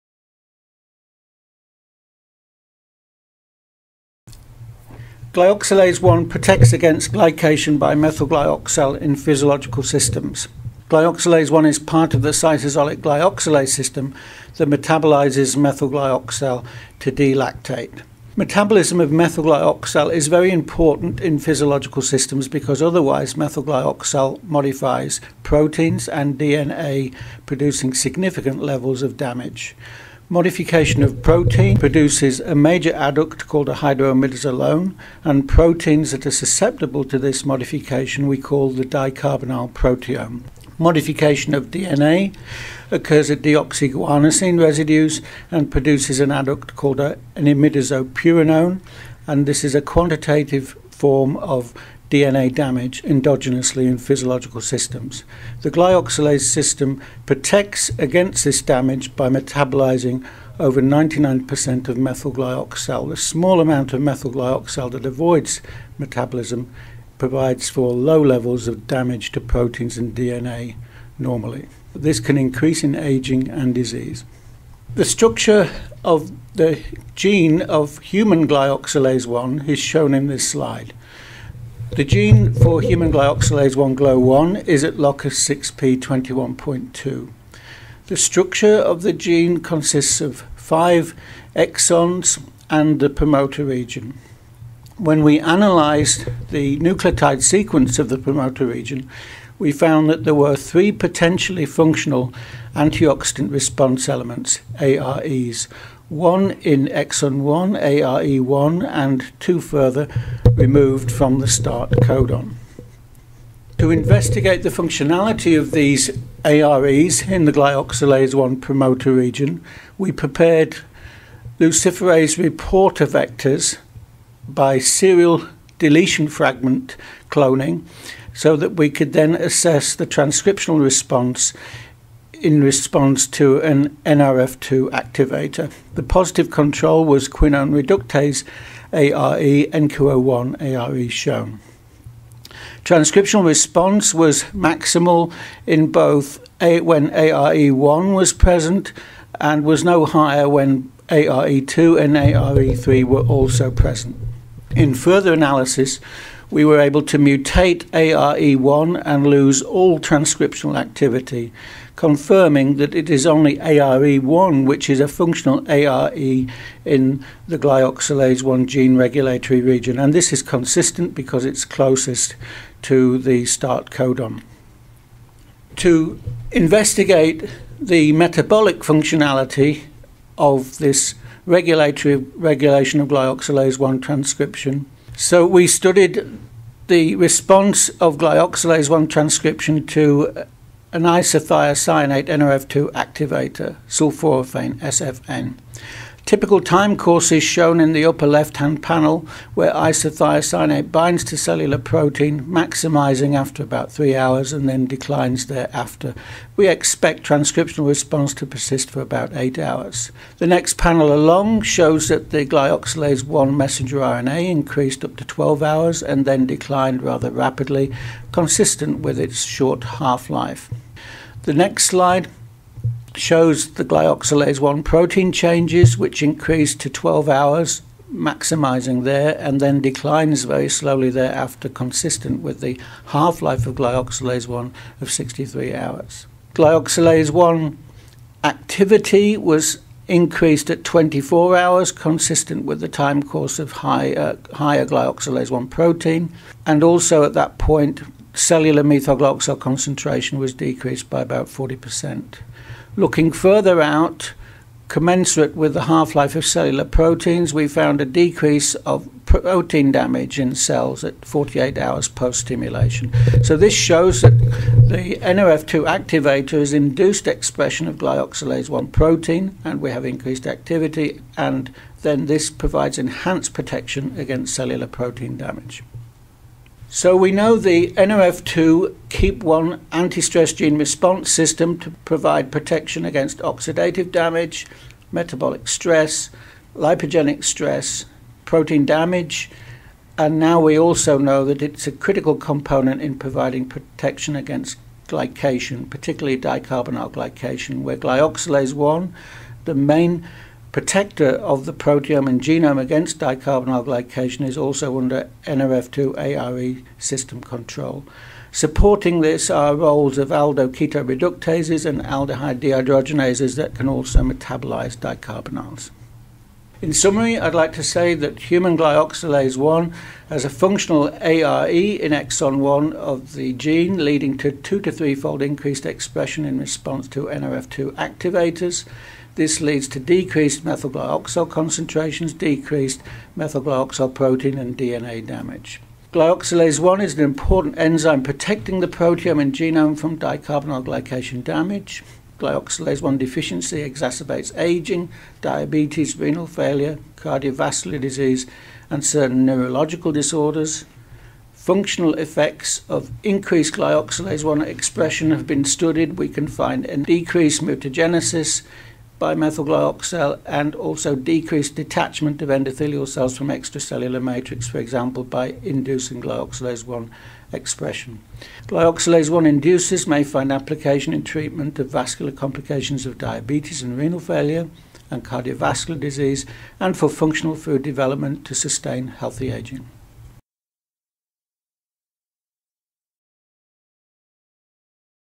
Hot Topic in Biochemistry: Transcriptional control of glyoxalase 1 by nrf2 by Paul Thornalley
Webcast of the presentation entitled 'Transcriptional control of glyoxalase 1 by nrf2 provides a stress-responsive defence against dicarbonyl glycation' given by Paul Thornalley  (University of Warwick, UK), presented at the Biochemical Society Hot Topic Event in December 2011.